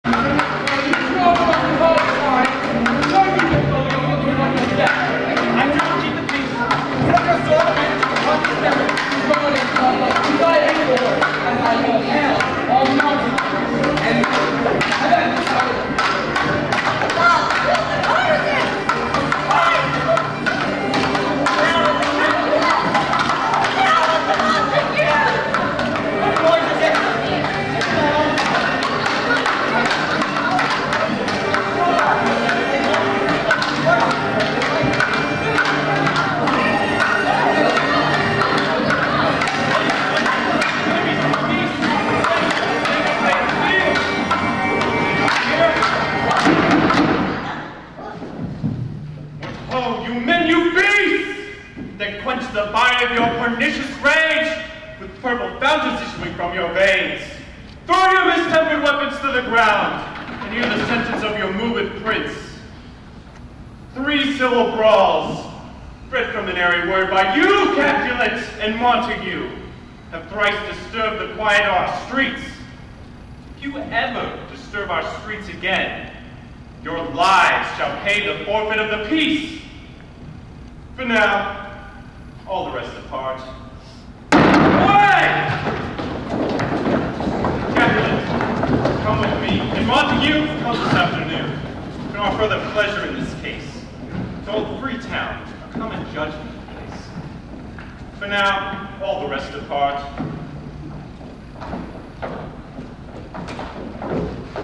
Romeo and Juliet Opening Scene
Loudoun County High School
May 21, 2016
Jake as the Prince of Verona